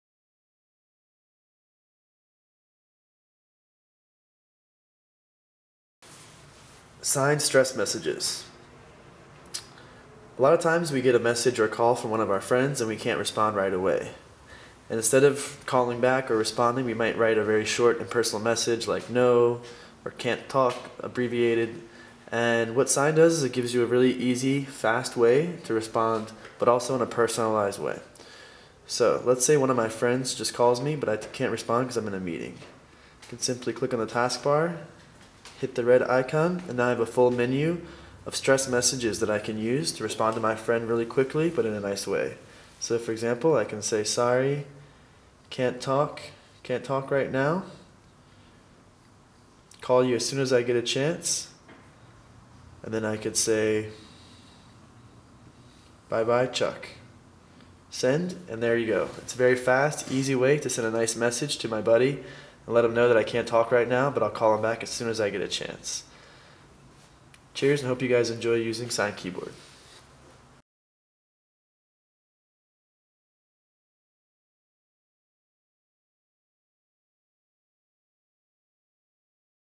Siine Keyboard Stress Messages Tutorial
How to use Stress Message Siines on Siine Keyboard.